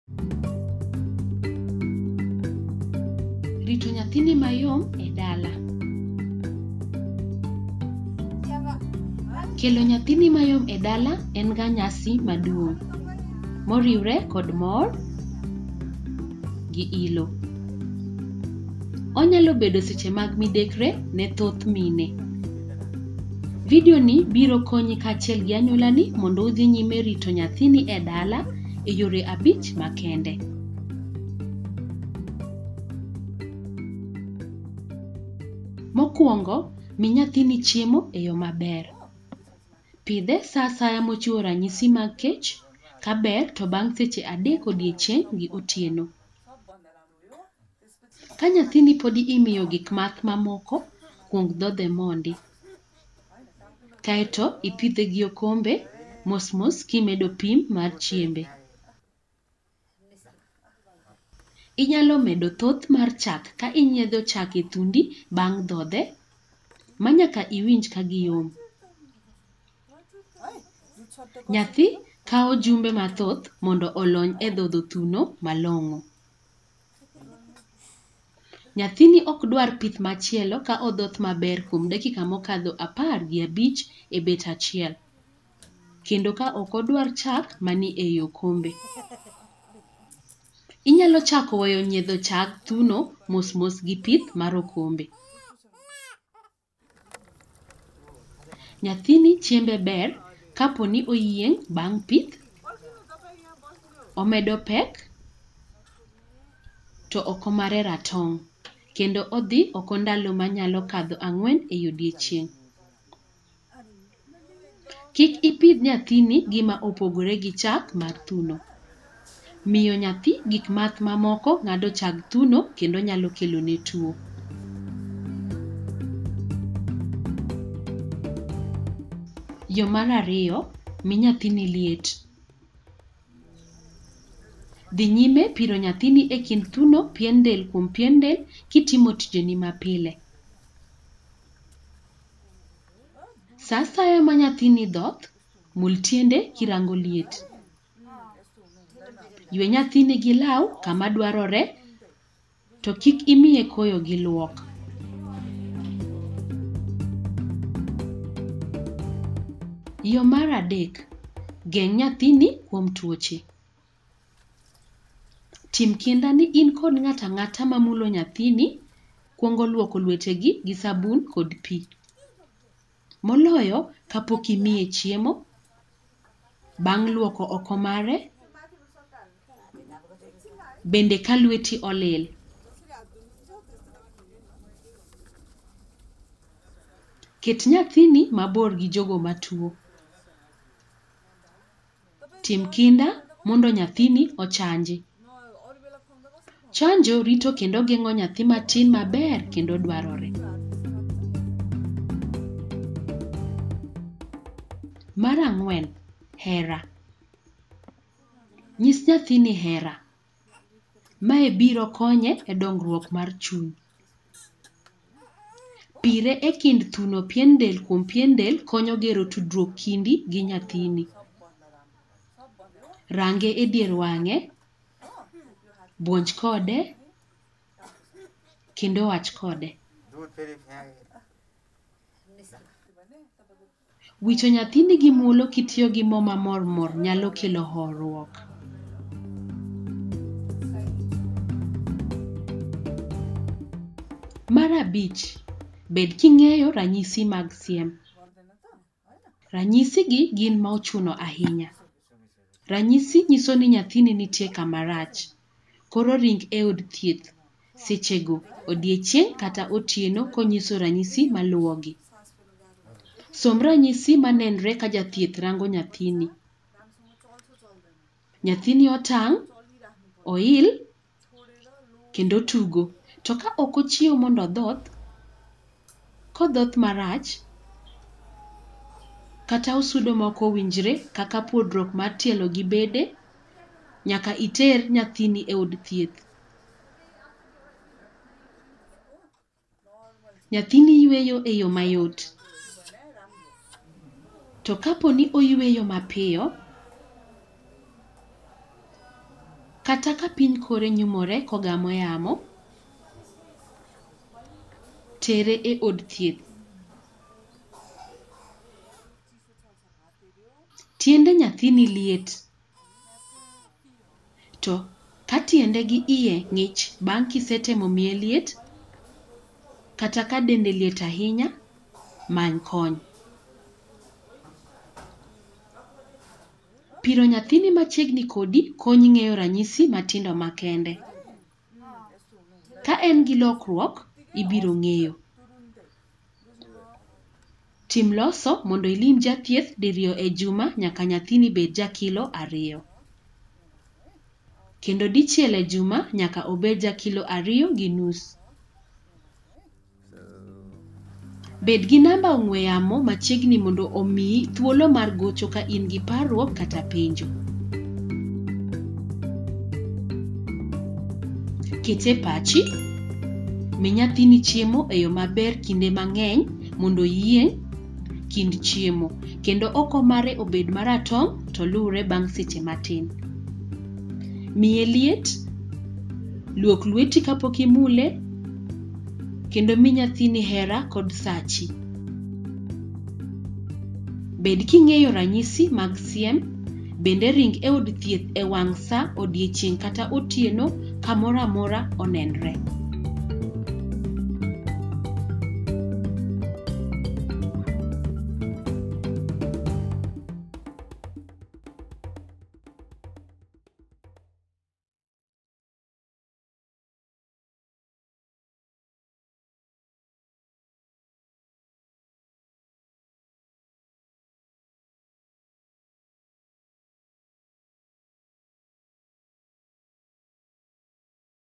Caring for Your Small Baby at Home (Luo) - Small Baby Series
By Global Health Media Project.

Bringing your small baby home can be a time of vulnerability for many mothers. This video shows you and your family five important ways to continue to care for your baby at home.

This video is intended primarily for mothers of babies who are born too small or too young in the developing world, but may be helpful to mothers of these babies worldwide.